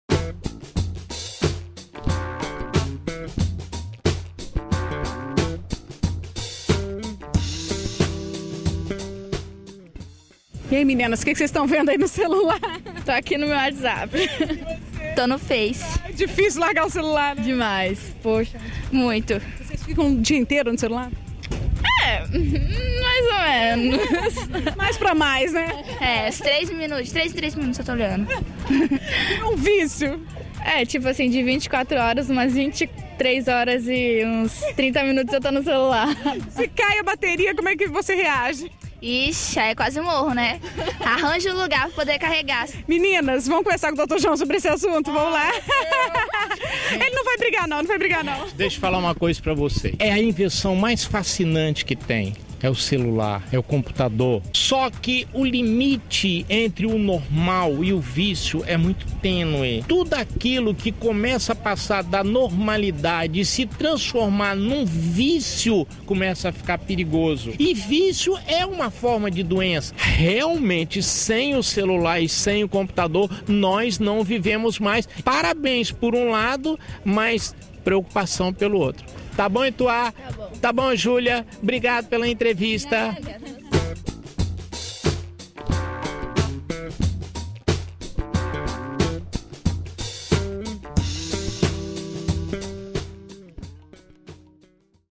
Dr. João responde
Te dá angústia passar dias ou até mesmo horas sem o celular? Sabia que esse vício tem nome? Nomofobia! É com você, doutor joão.